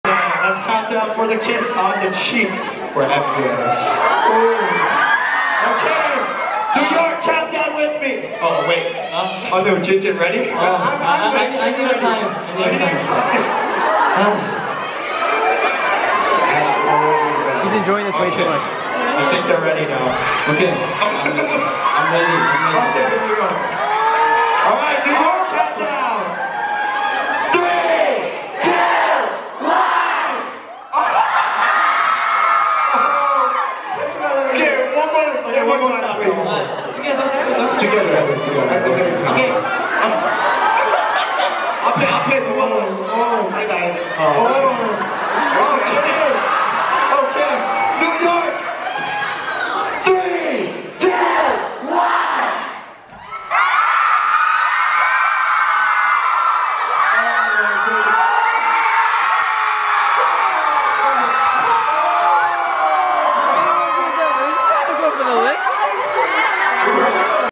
[ASTRO in NYC] Eunwoo & JinJin kissing MJ
Their team lost a game during the fanmeet and so they had to give MJ kisses on the cheek. It was cringy lmao